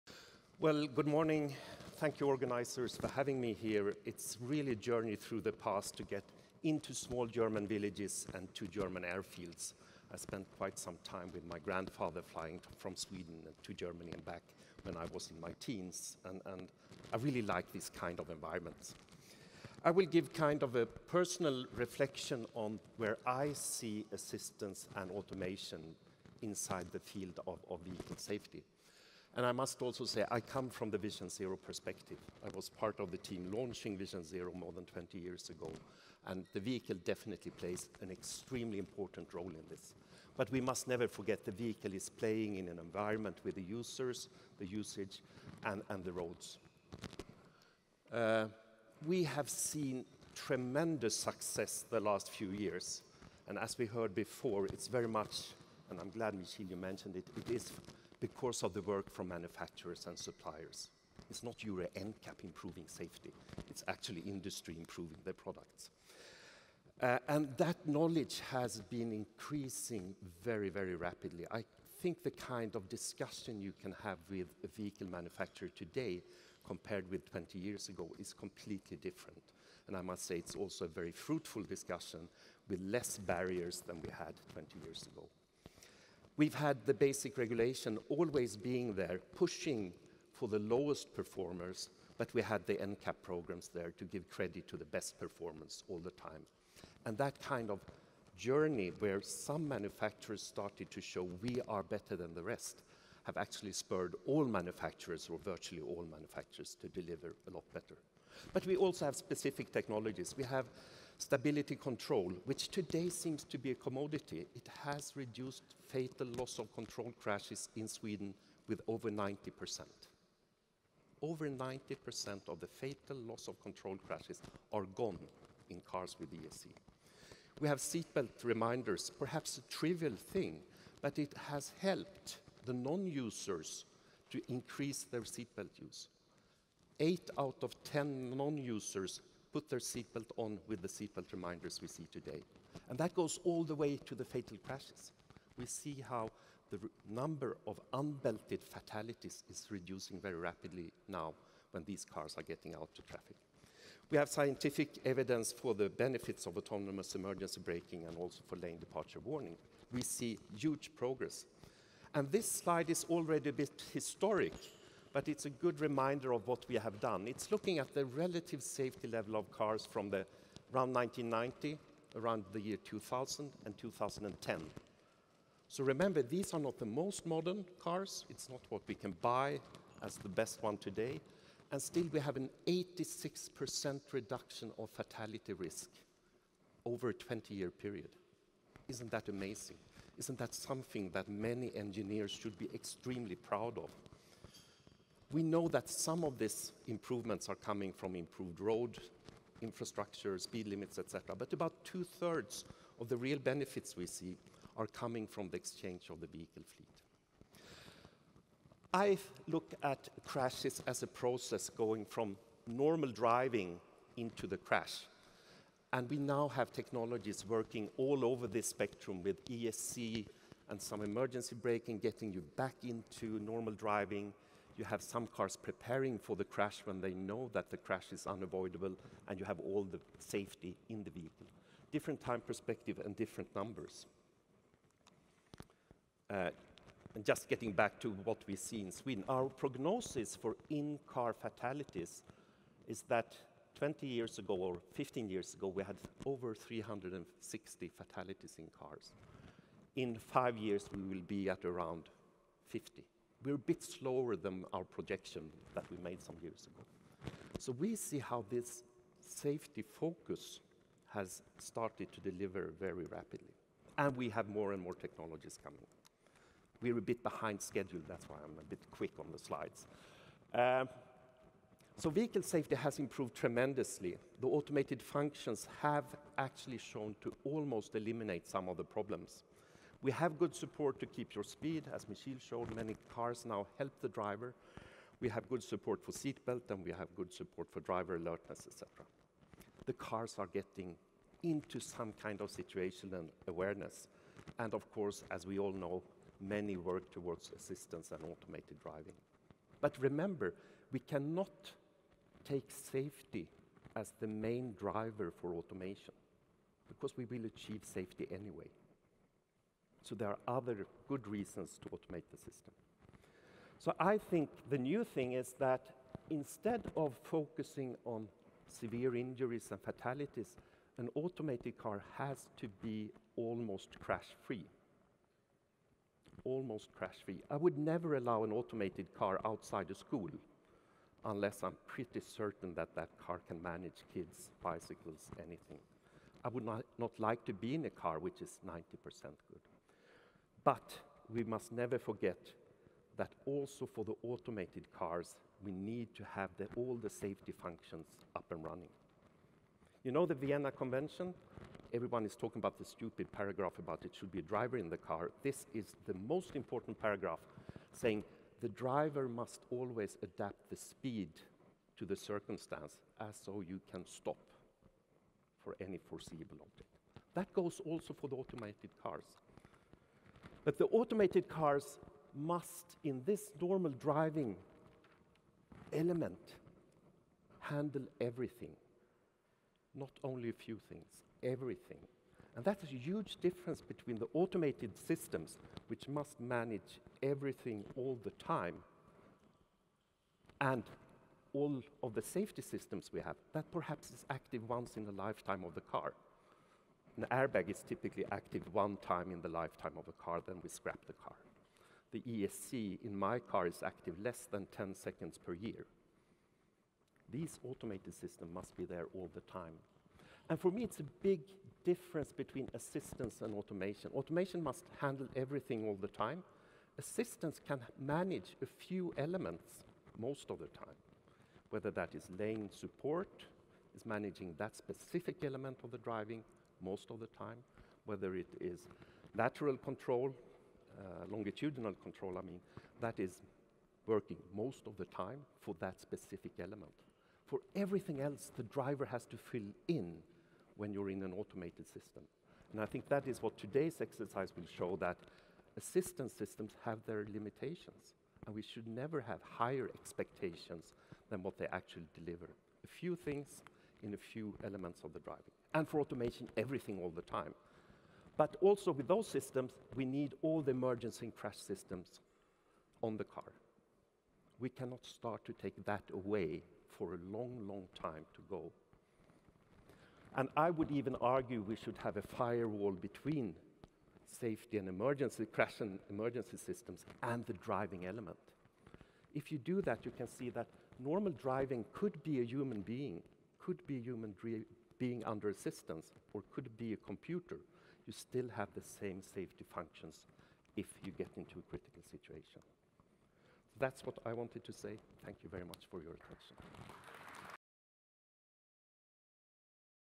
Assisted and Automated Driving - some Swedish reflections
Anders Lie, Specialist, Swedish Transport Administration, presenting at the Testing Automation launch in Landsberg on 18 October.